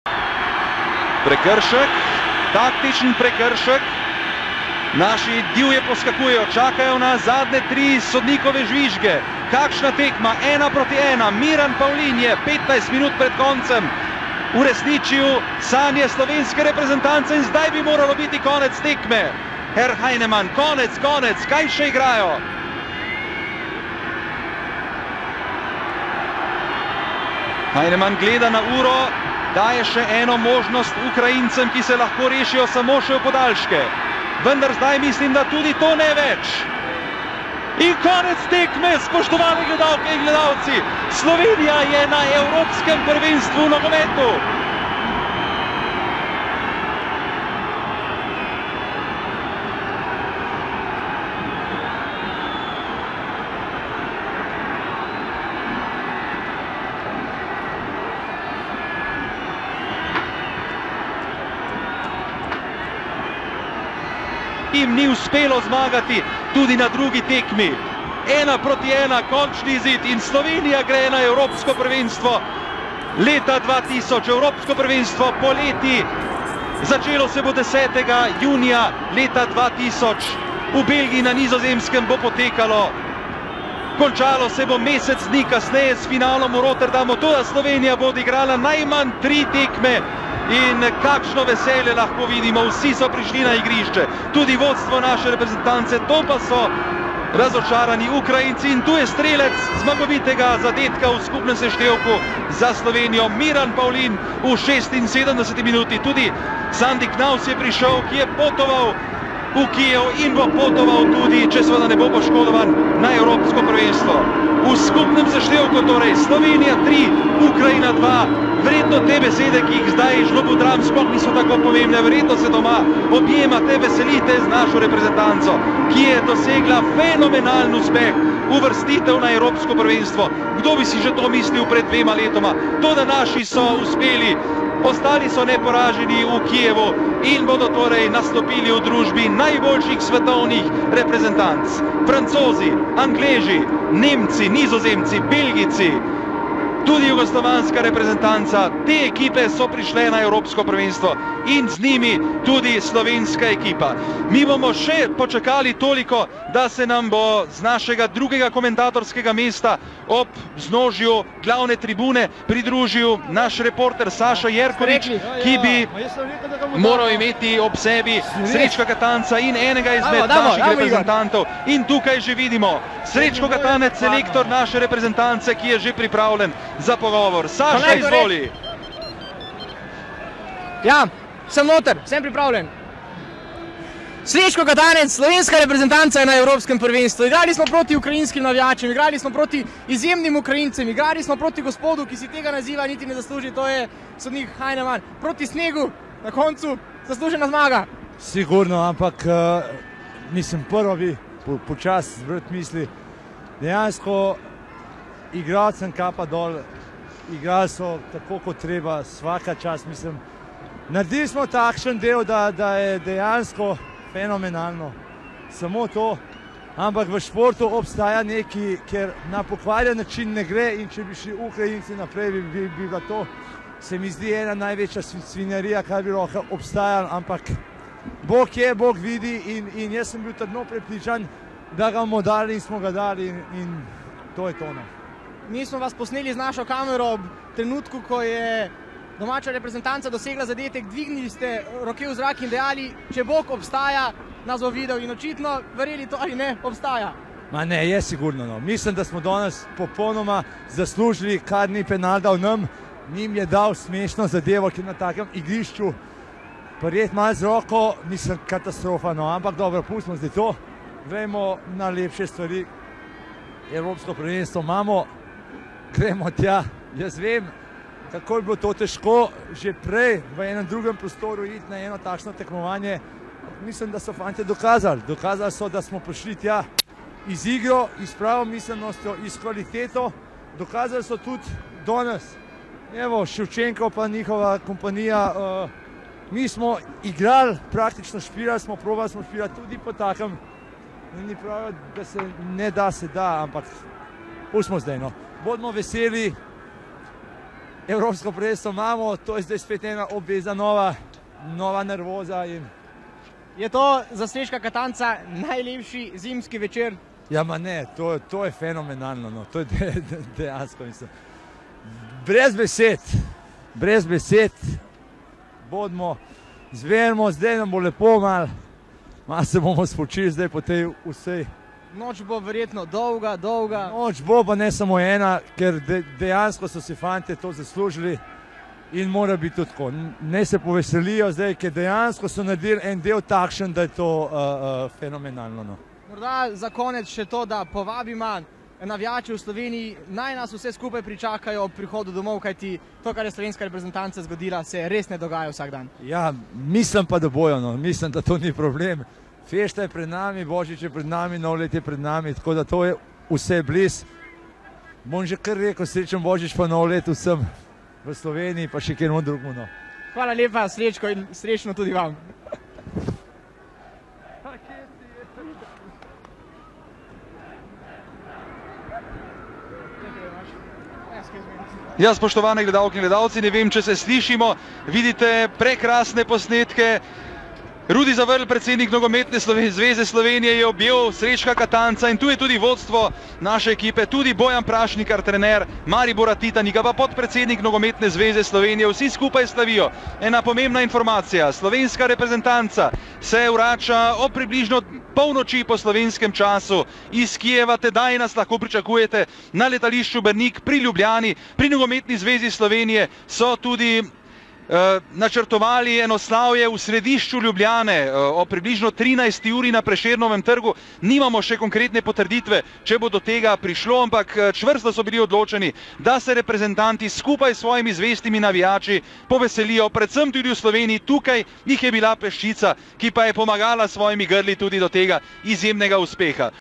Ukrajina 1-1 Slovenija (17. november 1999) - Konec tekme, Slovenija gre naprej!
Zadnji trenutki tekme Ukrajina - Slovenija, kjer so Slovenci s skupnim izidom 3-2 odšli na EP v nogometu 2000.

Nisem lastnik posnetka, avtorske pravice pripadajo pravemu lastniku.